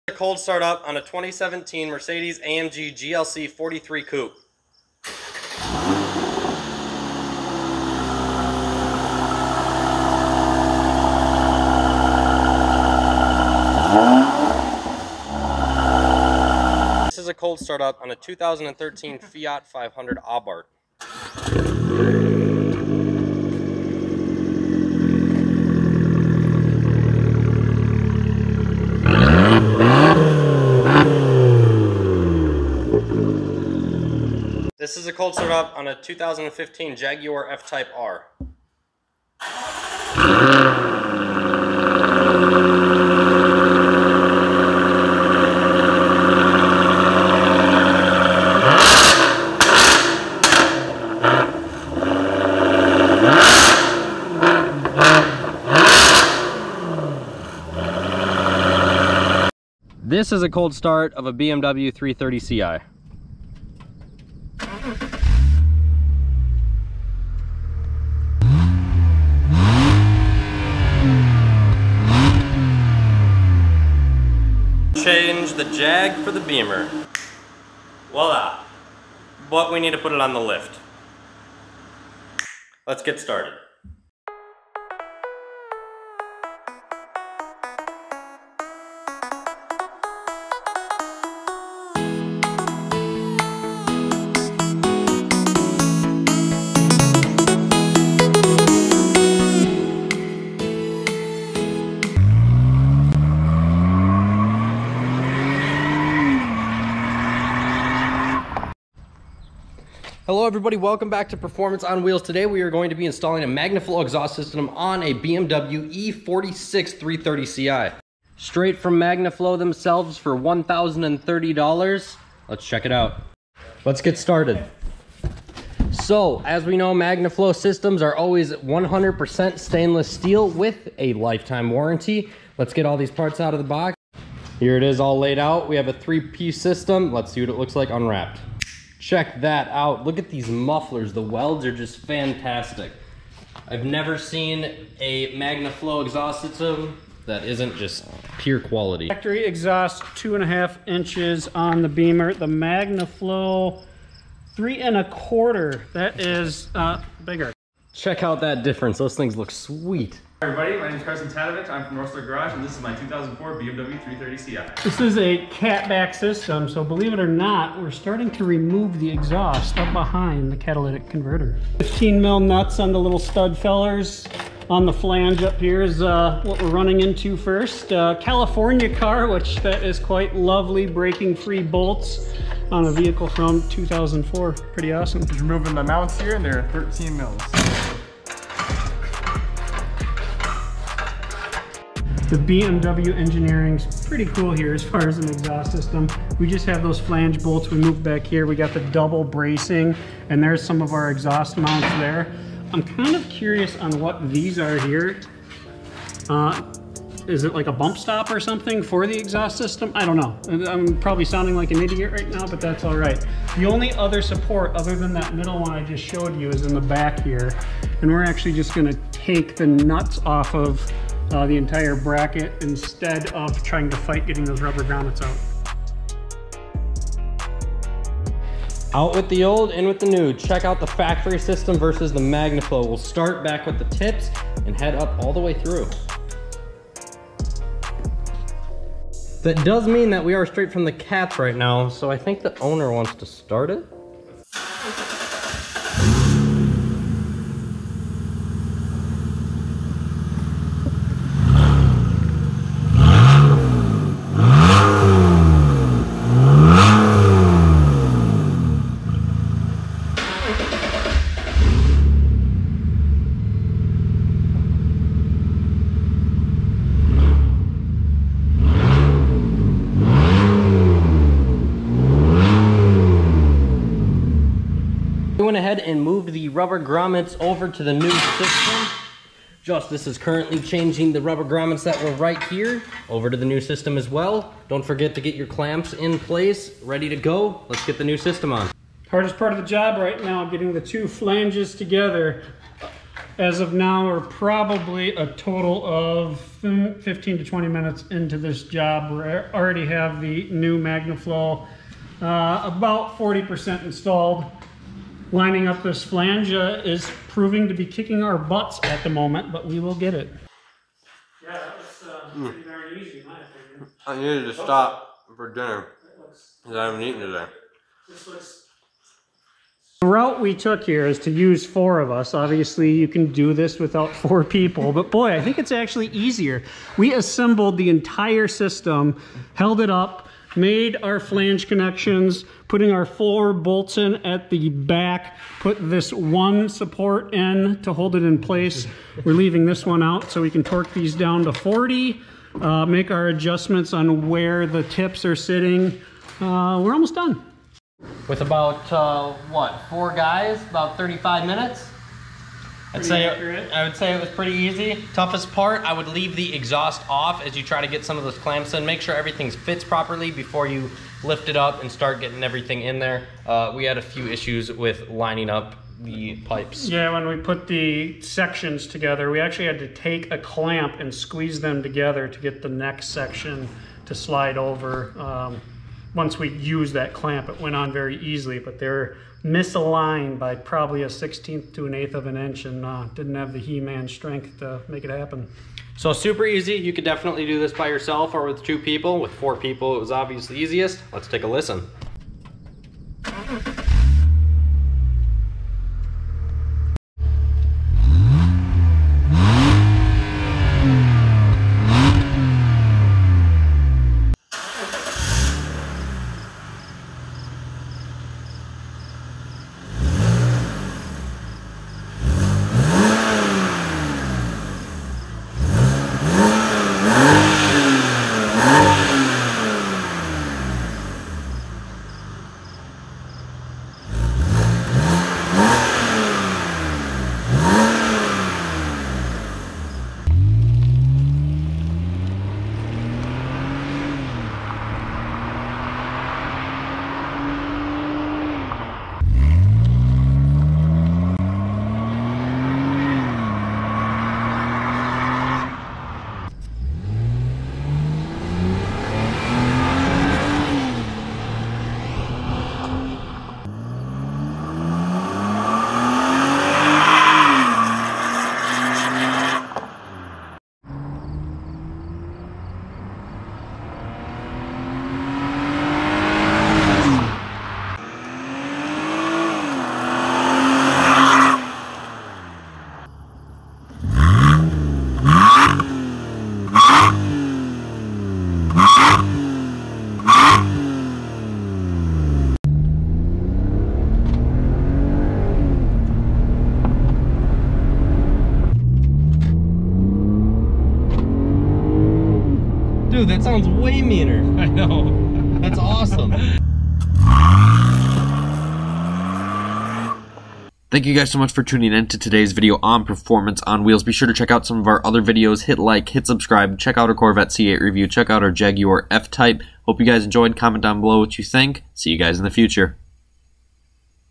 Installing Magnaflow Exhaust On A BMW E46 330 CI
Join us today on Performance On Wheels as we install and check out a Magnaflow Exhaust system on a 2004 BMW E36 330CI!